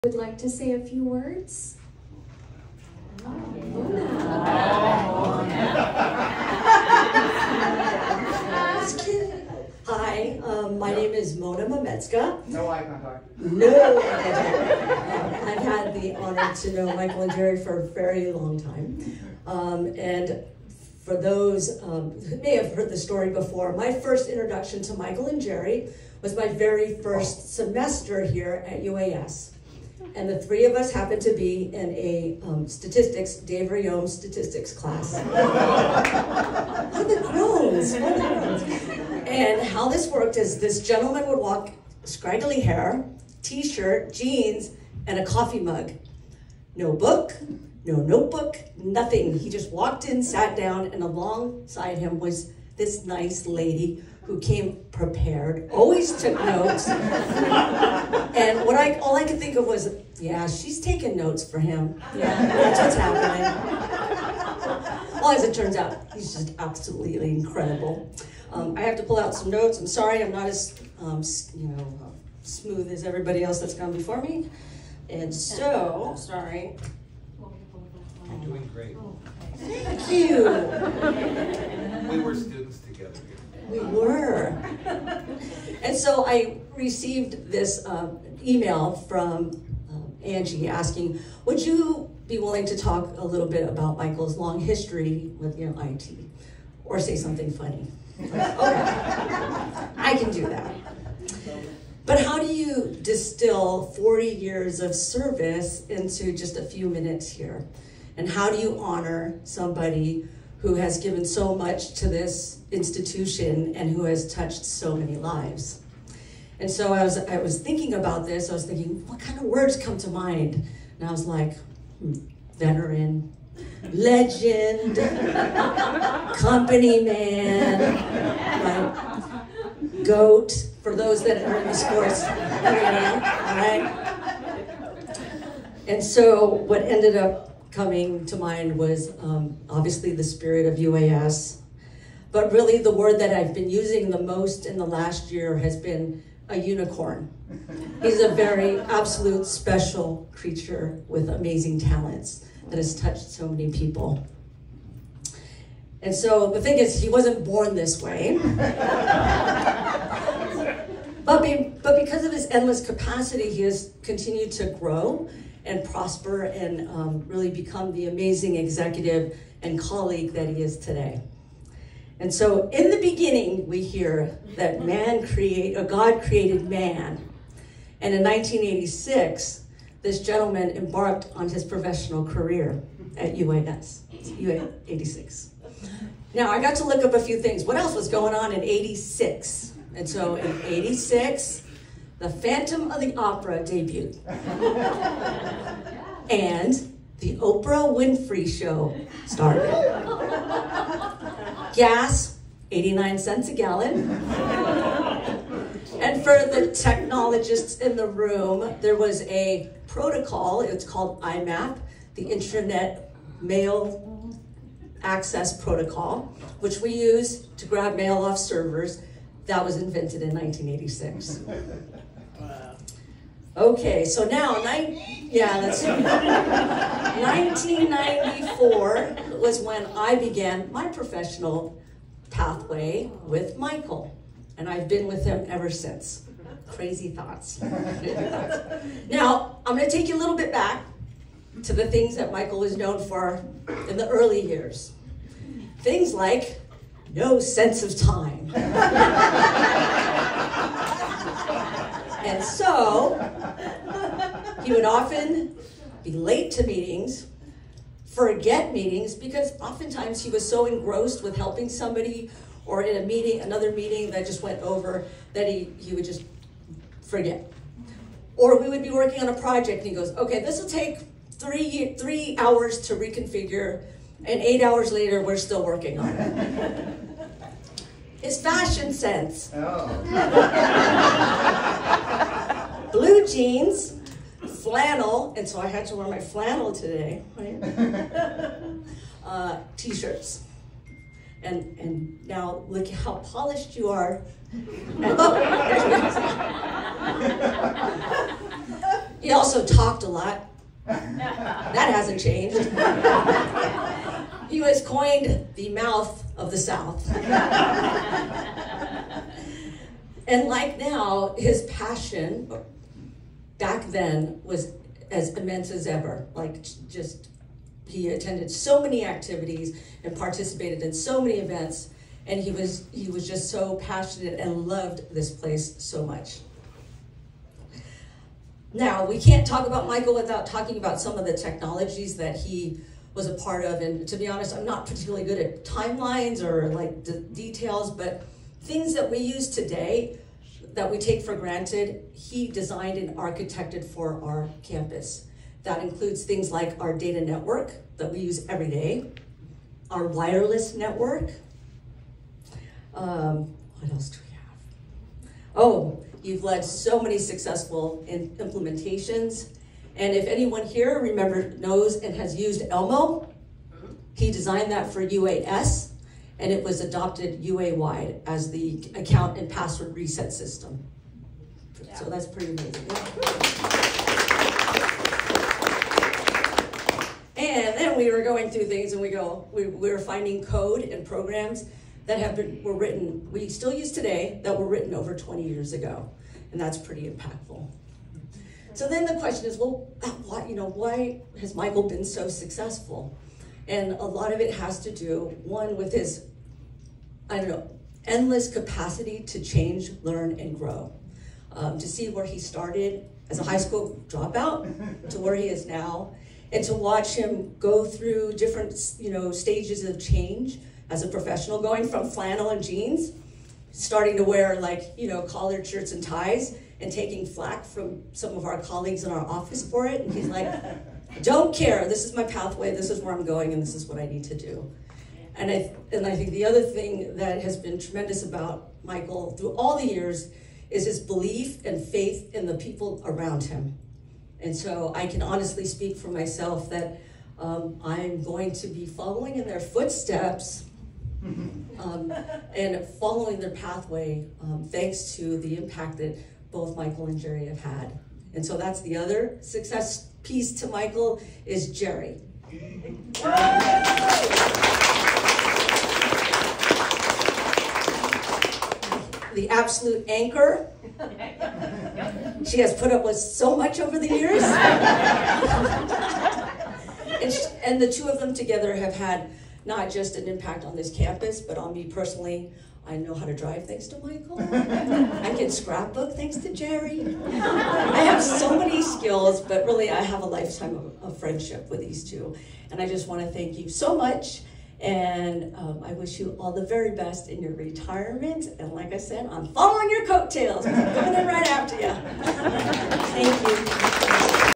Mona Mametsuka, UAS Information Systems Mgr. address @Michael Ciri and Jeri Cary's Retirement Party
Longtime friend and co-worker Mona Mametsuka, UAS Information Systems Mgr. address @Michael  Ciri and Jeri Cary's Retirement Party 9/29/23 8:14 minutes. Funny! She calls Michael: a Veteran, Legend, Company Man, Goat and best of all, a very special unicorn.